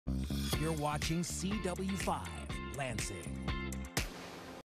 WLAJ-DT2 Station ID (2024)
This is WLAJ-DT2s station ID from 2024
WLAJ is located in Lansing, MI